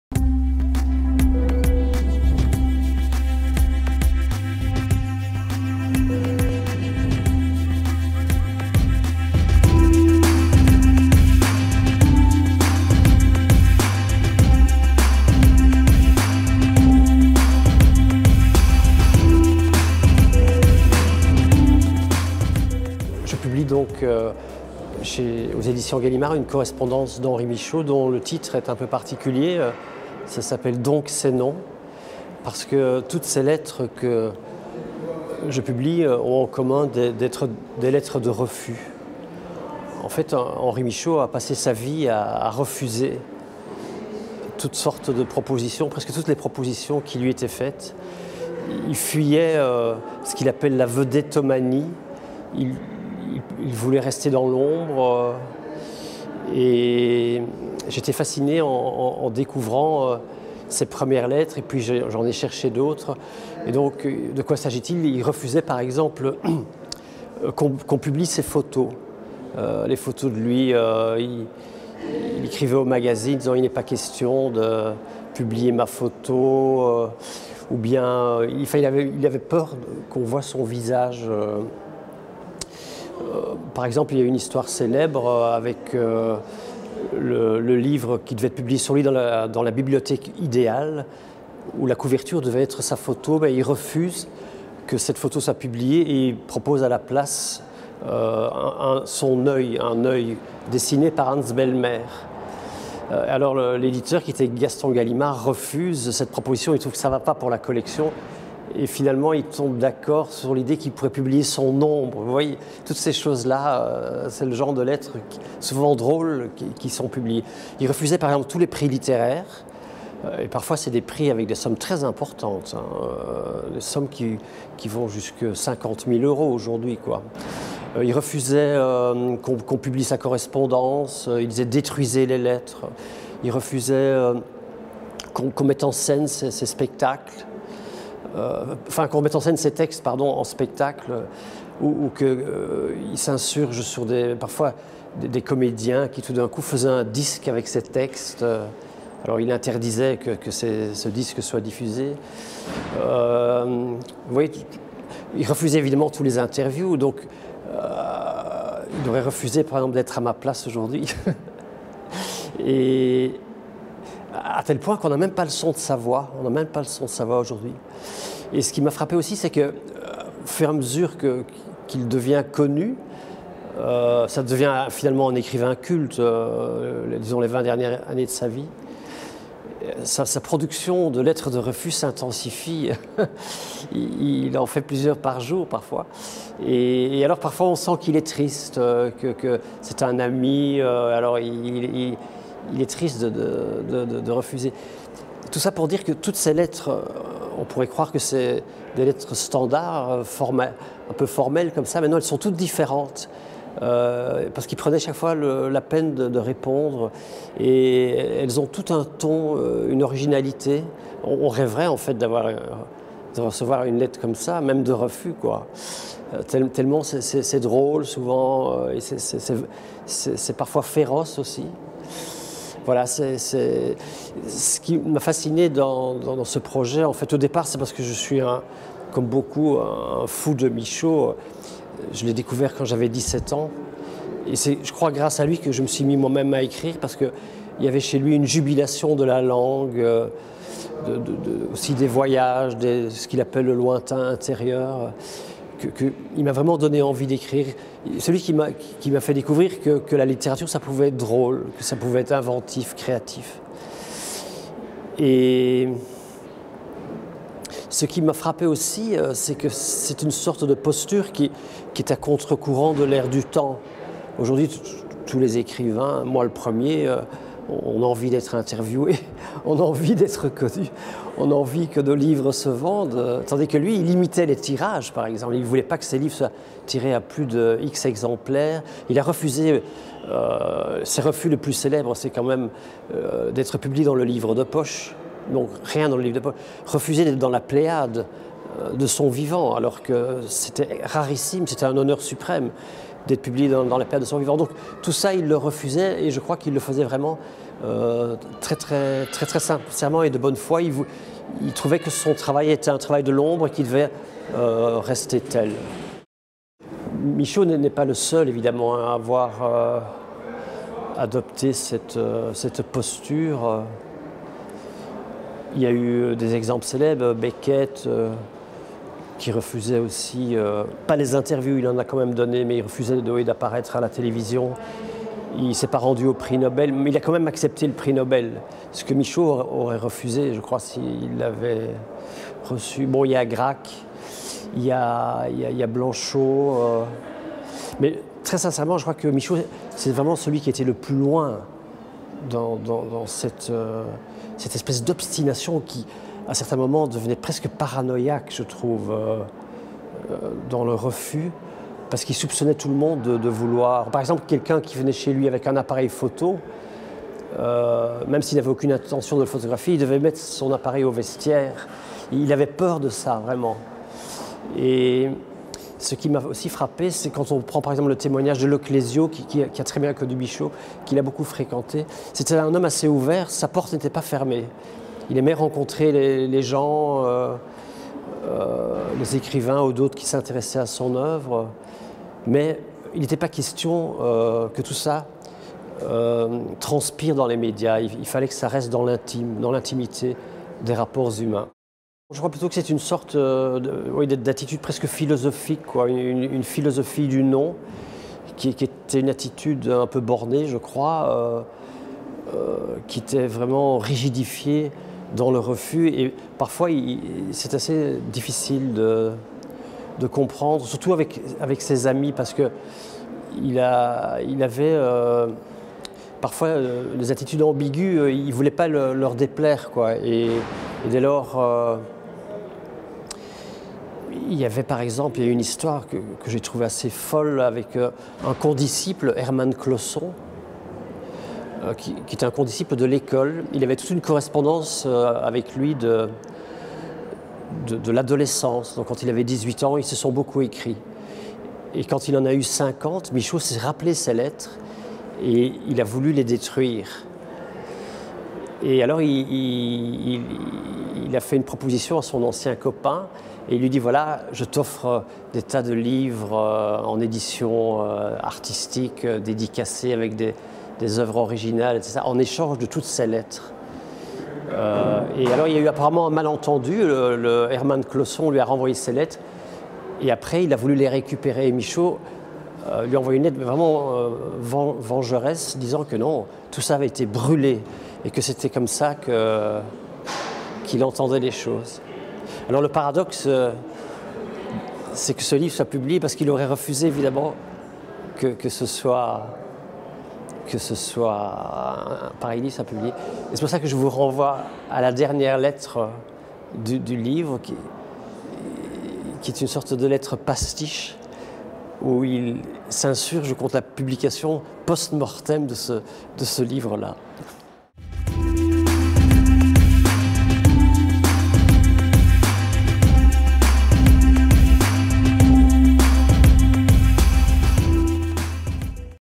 Jean-Luc Outers - Donc c'est non
Jean-Luc Outers vous présente "Donc c'est non" aux éditions Gallimard. Auteur : Henri Michaux. Lettres réunies, présentées et annotées par Jean-Luc Outers.